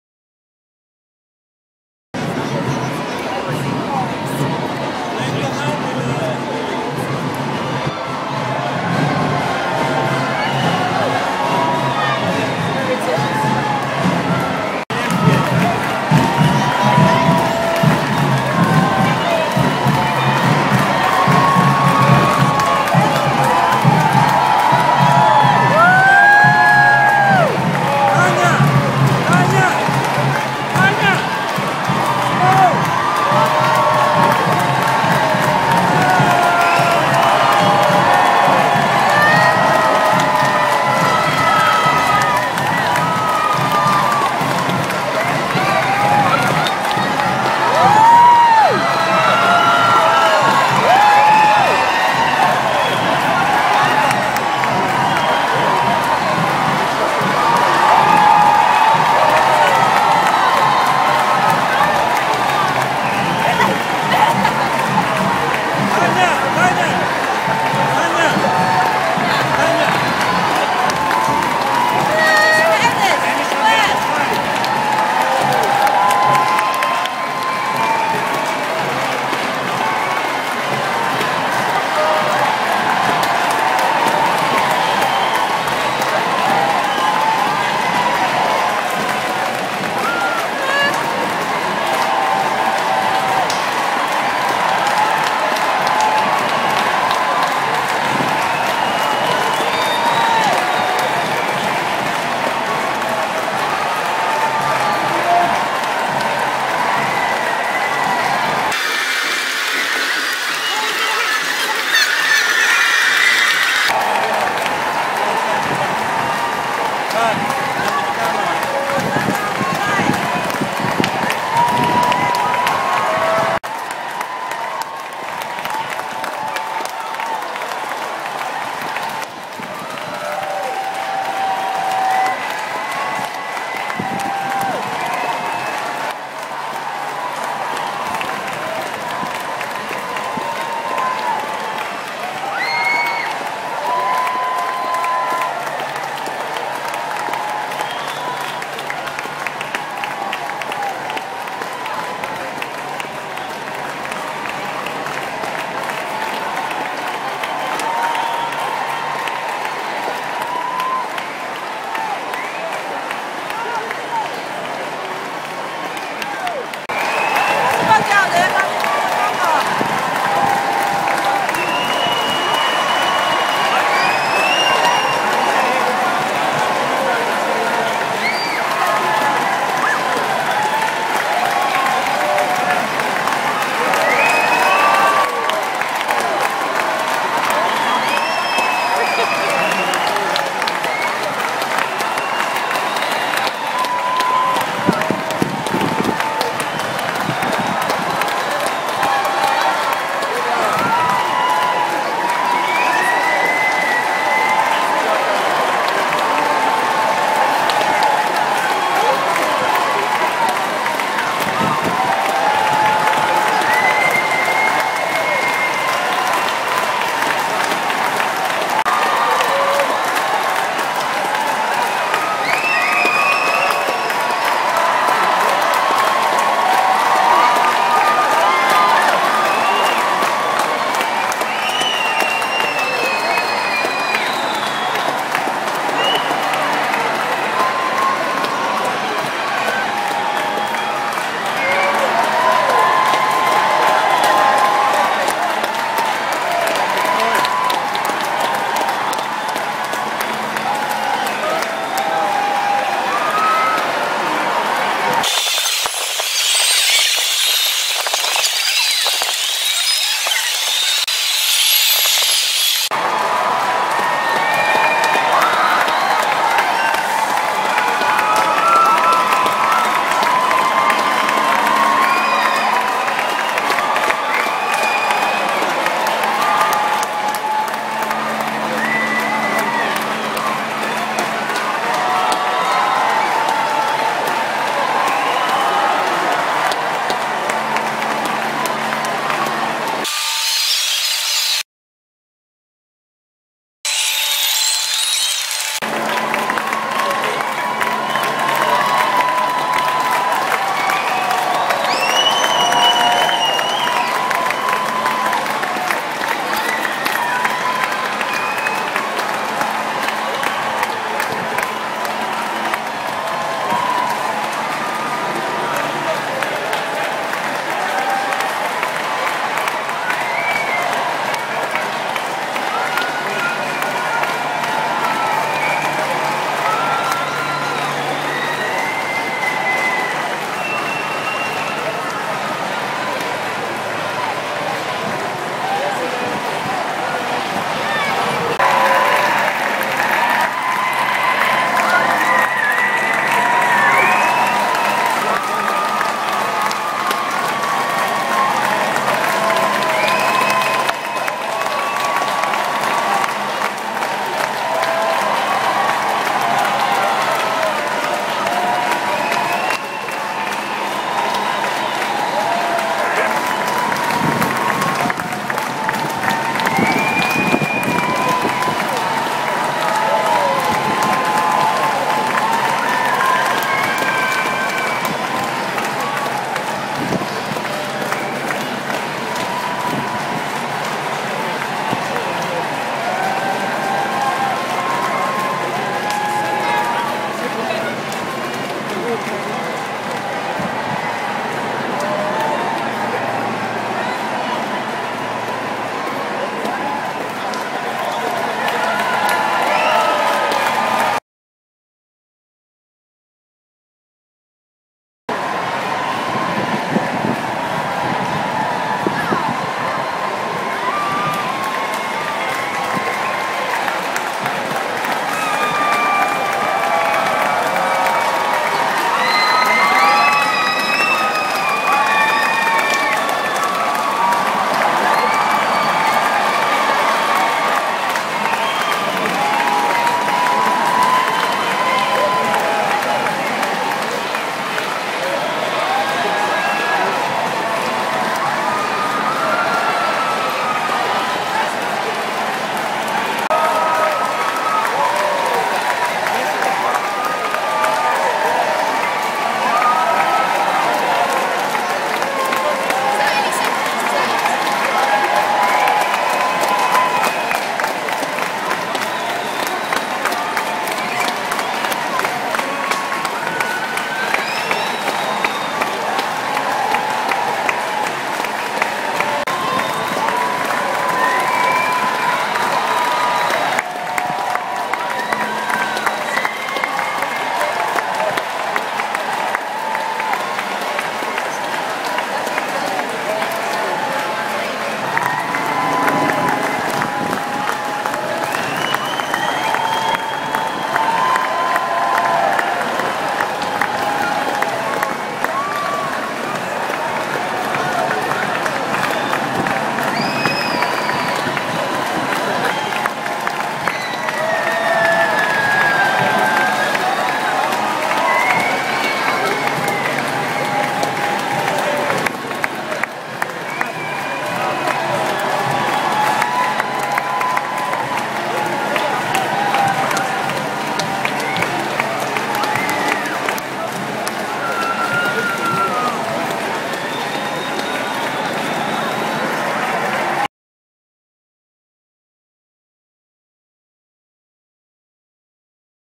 Athletes' Parade London 10th September 2012
Parade of our fantastic Olympic and Paralympic athletes in London on 10th September 2012.   Sorry for the shaky camera work.